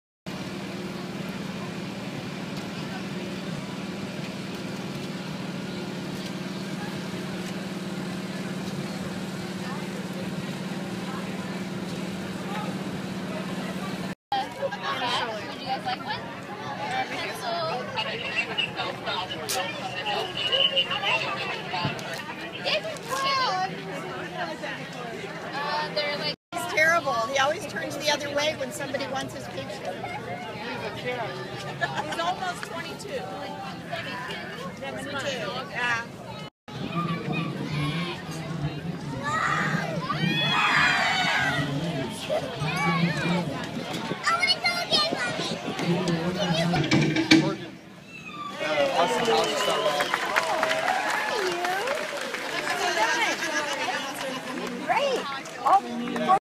The street fair is underway for the 4th in Old Town.
By: Shaun Walker
Published on: July 4, 2014